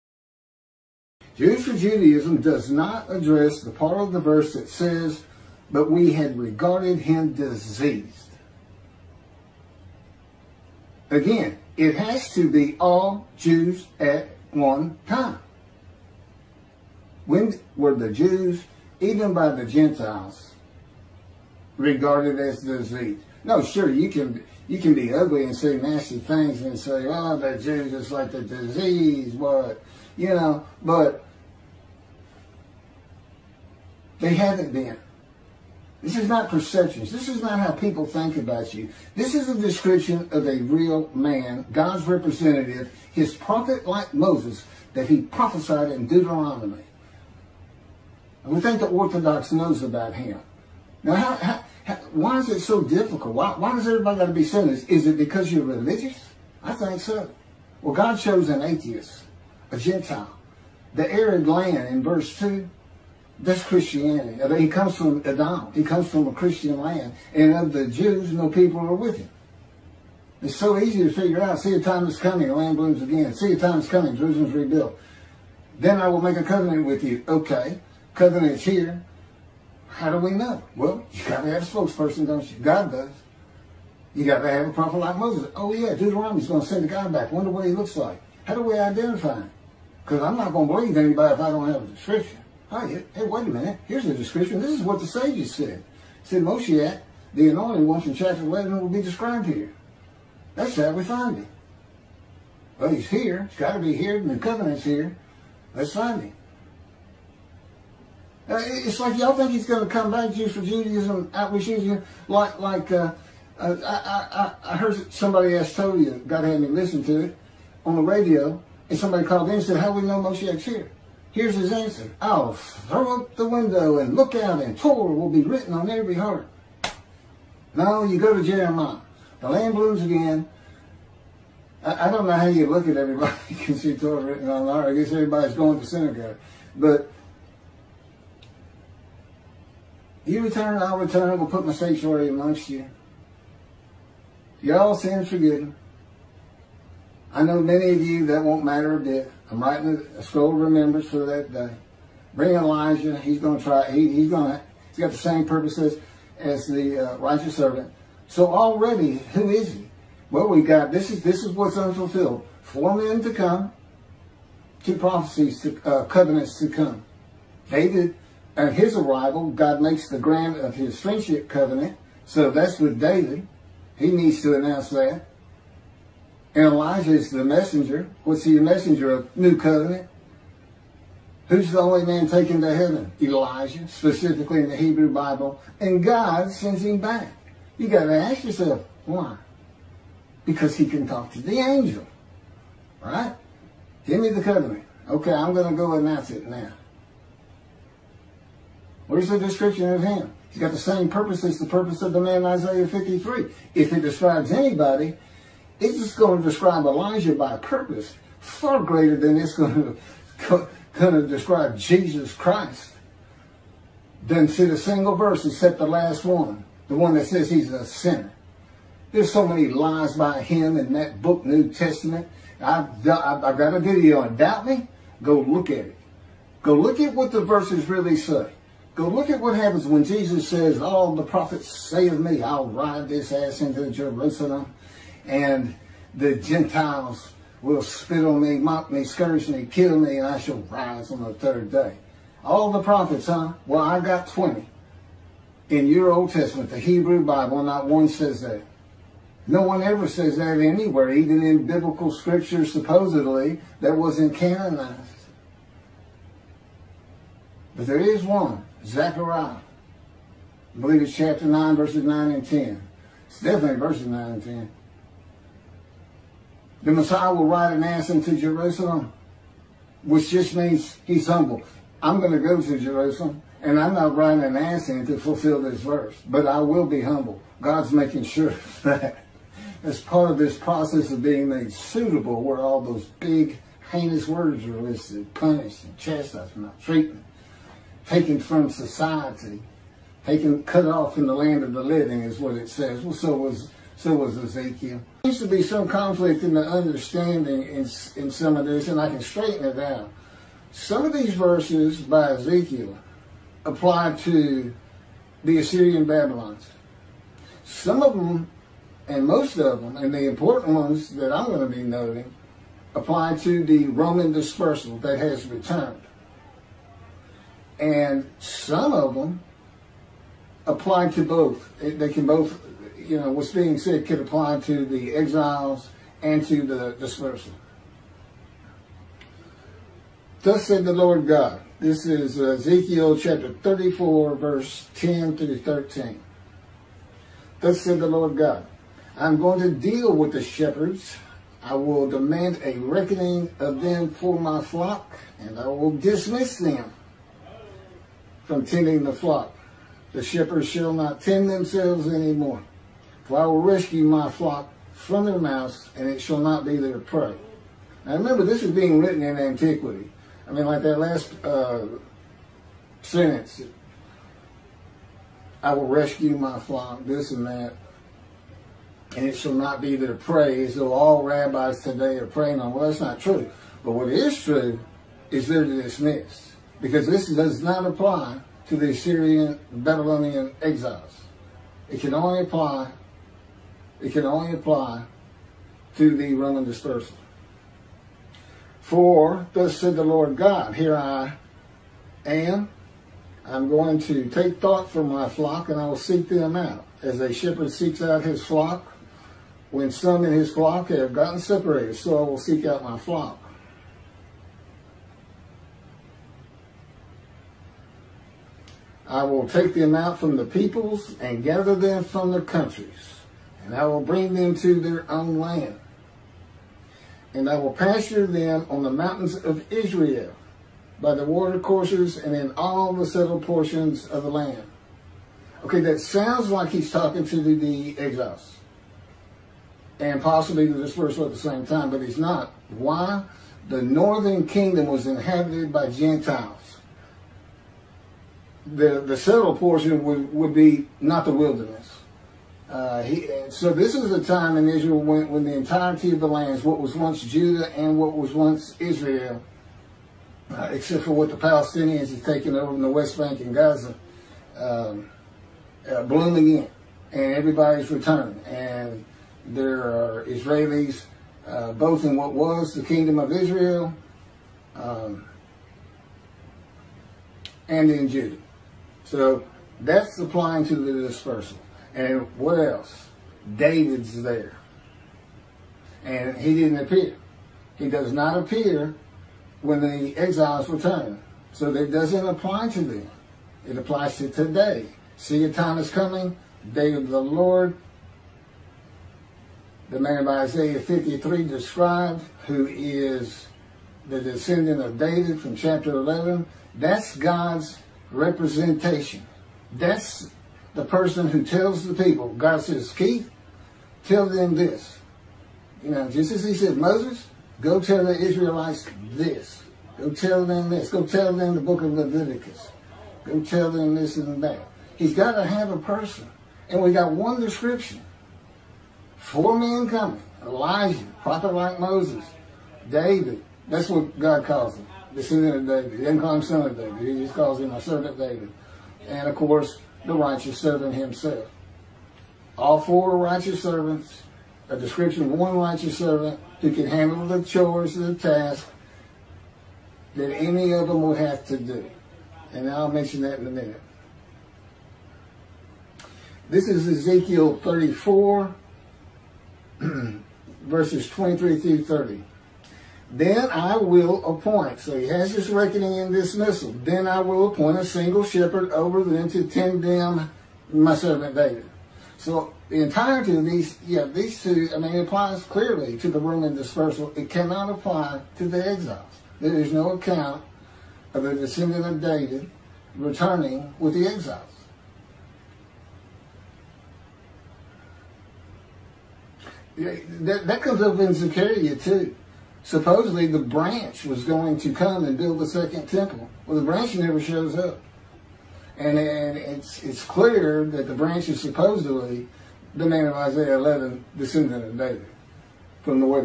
The Day Of The Lord Is Here It Begins With Jerimiah 31 When God Says “see a time is coming" 3 Times
Isaiah 53 describes the man who will be God’s representation, His prophet like Moses speaking and writing His words to the Jewish people, just as He did with Moses and the Israelites, in this day of the Lord. The day described in Malachi 3 where He says He is returning with His messenger and the Angel of His presence the Holy Spirit. He is Moshiach of Isaiah 11 known as the leper scholar in the Talmud for he is familiar with disease and makes the many righteous with his knowledge.

Sages, Rabbis & Christians have never figured Isaiah 53 out. Only God’s righteous servant can interpret it properly. God had it written that way. He leaves something out that can be seen in Ezekiel (the key to 53) to make his forehead like adament, flint, and to not be dismayed by his people. He went through God’s fire of refinement pinned to the ground and taken from the land of the living (society) after God's Spirit entered him, and God is in His Spirit.

God’s fire of refinement for prophets of wounding, punishment, chastisement, maltreatment, crushing and bruising to break the will of a man, tempering his emotions and removing a furious spirit while teaching him the scripture.

In my case to become His righteous servant and make the witnesses of the combined first six verses by quotation righteous. They are sick with guilt from sinning. I offered myself for guilt agreeing to the fire of refinement. So, when the witnesses say "he was wounded for our sins" I was. Not as a vicarious taking of their sins but to endure the fire of refinement and become the righteous servant who makes the many righteous by his knowledge and removes their guilt.

How We Know The Day Of The Lord Is Here?

It begins with Jerimiah 31 when God says “see a time is coming” three times. Summarizing the lands bloom again and the cities are restored; Jerusalem is rebuilt to at least its size in antiquity; and God will make a new covenant with His people of sin forgiveness that writes Torah on their hearts as they return to observant Judaism.

The land laid desolate for over two thousand years after the Roman dispersal until 1948. The Jews have returned and the lands bloom, cities have been restored, and Jerusalem rebuilt. The new covenant is here. God has returned with Moshiach and the Angel of His Presence the Holy Spirit.

Where is the new covenant and covenant of friendship & who delivers them to the Jewish people?

There are only two undelivered covenants. Friendship in Ezekiel 34 & 37 that comes with Moshiach and Sin forgiveness of Jeremiah 31. This takes you to Malachi 3/1 where God says He is sending His messenger (Elijah) to clear the way for Him and He will return to His Temple and the angel of the covenant that you desire is already on the way (sin forgiveness).

And the angel of the covenant that you desire is already on the way. That would be the covenant of sin forgiveness. This is the chapter God announces the day of the Lord. See a time is coming is the coming and now arrival of the awesome, fearful day of the Lord.

Elijah , God and the Angel of His Presence the Holy Spirit are here.

Who does the angel give the covenant to for delivery?

The messenger Elijah who is the only man in the Hebrew Bible specifically taken to Heaven and returns in the day of the Lord.  He would know the angel of the new covenant who is the Angel of God's Presence the Holy Spirit.

How will we know who he is?

There are still three men to come in the Hebrew Bible who are Moshiach, Elijah, and the prophet like Moses. Each of these great men were righteous and all three were servants of God. One more man to come is Gods righteous servant of Isaiah 53. Four righteous servants to come and a description of just one and that means he has to be all four. The Sages knew you had to have a description of Moshiach and it was Isaiah 53. They called Moshiach the Leper Scholar.

Who does not fit every verse to be God’s representation in this day of the Lord, will clear the way for Him by having the third Temple built for His return to dwell amongst His people forever, and deliver the last two covenants?

Jesus and the Jewish people as “Israel” are not even in the conversation.  It is a man who fits every verse who must live in a gentile country because God comes from Edom and who must be a gentile because of the Jewish people none are with God.

I am a descendant of David by the anointment of many tasks with God's sprit alighting upon and entering me. As they did with Ezekiel bearing the punishment (sins) of the houses of Juda and Israel.

God is telling me every word you just read and this channel site is every chapter of two Books He dictated two me including how I fit every verse of Isaiah 53. These are my proofs of who I am.

Please share every video you watch with all of the Jewish people you know and all Jewish groups where it is acceptable to do so. This is the third Temple era. There is no messianic era taught by Rabbis. A falsehood  & Isaiah 53 being Israel.